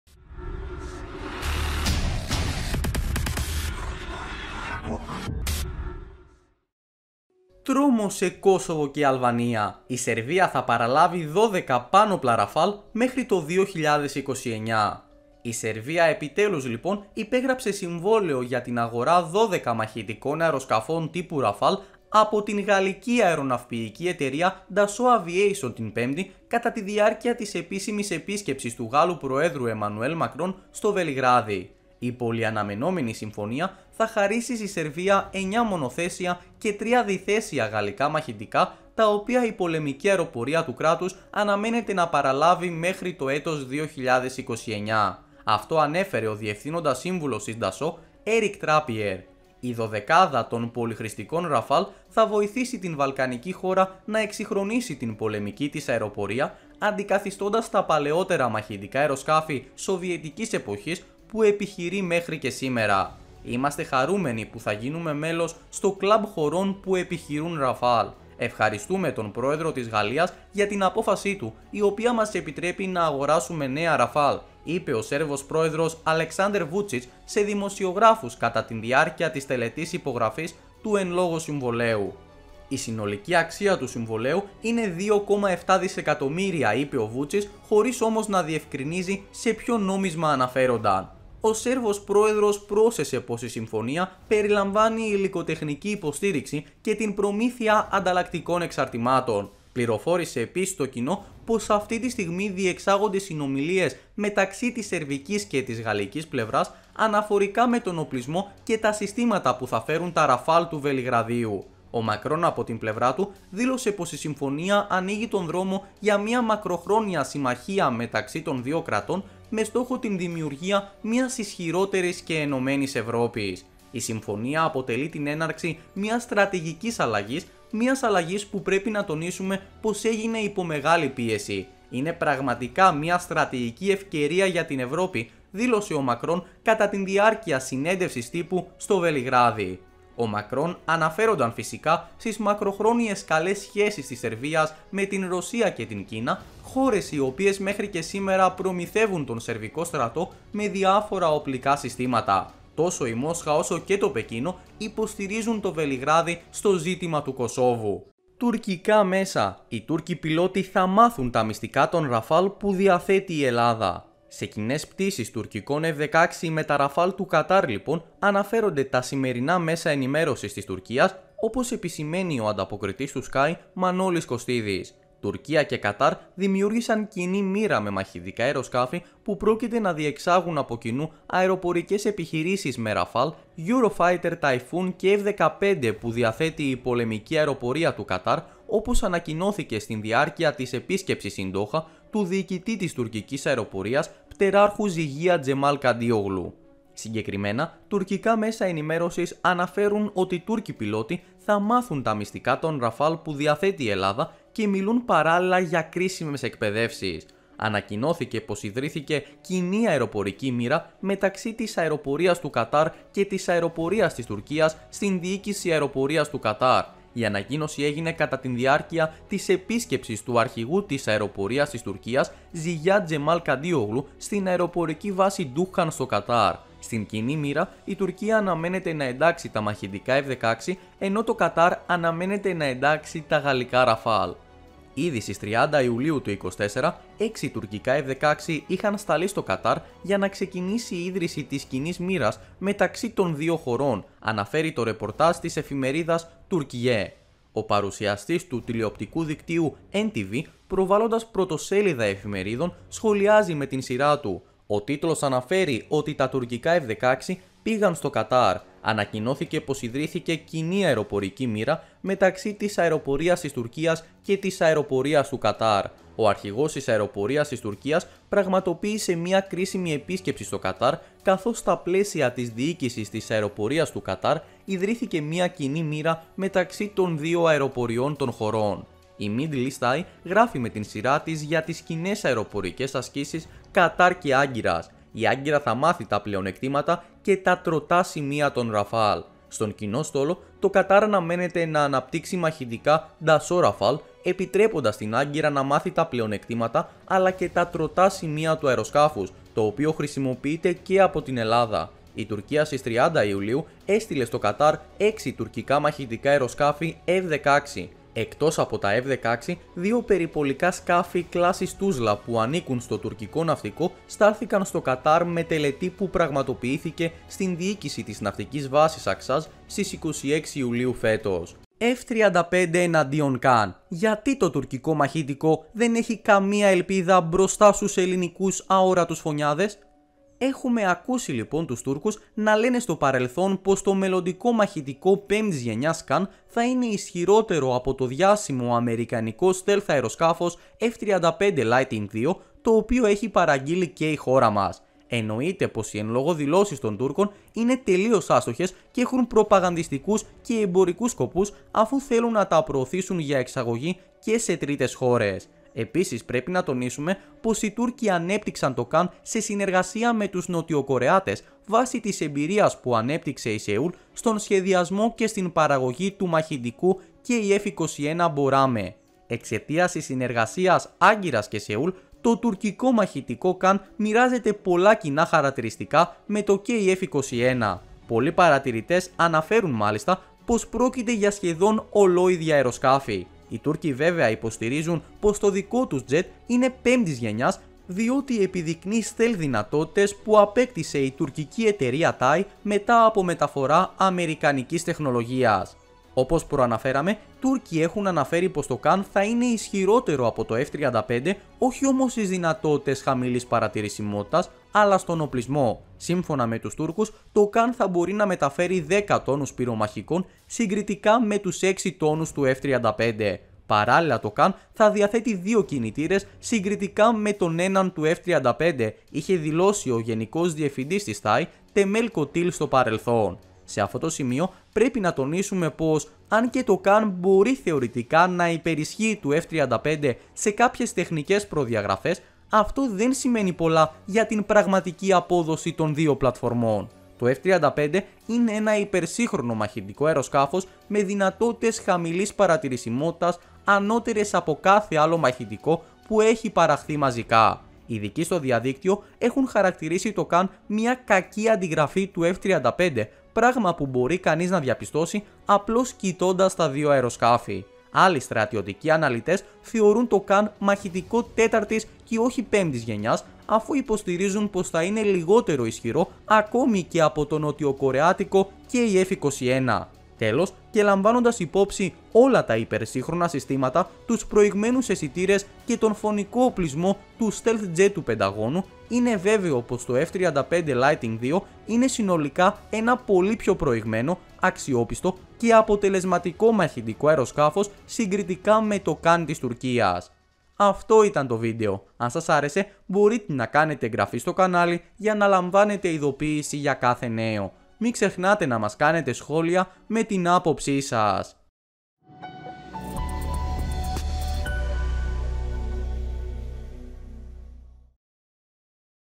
Τρόμος σε Κόσοβο και Αλβανία! Η Σερβία θα παραλάβει 12 πάνοπλα Rafale μέχρι το 2029
Τρόμος σε Κόσοβο και Αλβανία από το νέο απόκτημα του Βελιγραδίου

Η Σερβία θα παραλάβει 12 πάνοπλα Rafale μέχρι το 2029

Ο Σέρβος πρόεδρος Βούτσιτς ανακοίνωσε πως το συνολικό κόστος της πολυαναμενόμενης αγοράς θα φτάσει τα 2,7 δισεκατομμύρια

Κεφάλαια:
0:00 - Εισαγωγή
01:03 -Οι Δηλώσεις Βούτσιτς και Μακρόν
02:38 - Οι Τούρκοι μελετάνε τα Rafale
05:21 - F-35 vs KAAN